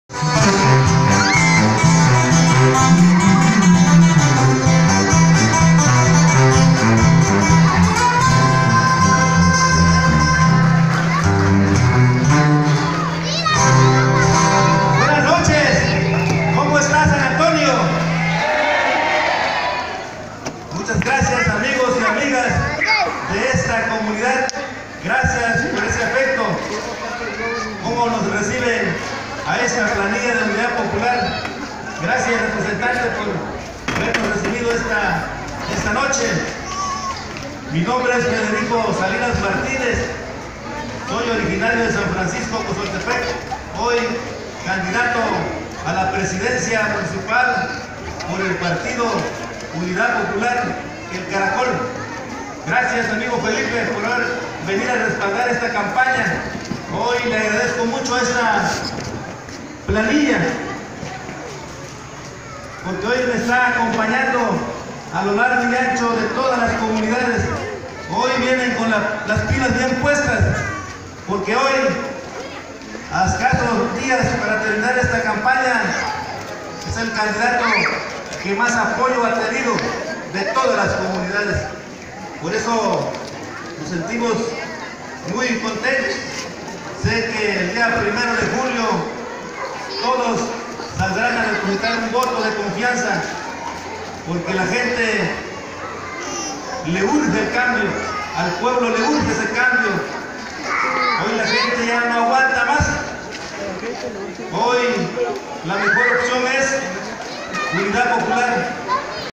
lico salinas san antonio tonameca
Federico Salinas, candidato a la presidencia municipal de Santa María Tonameca realiza pre cierre de campaña en San Antonio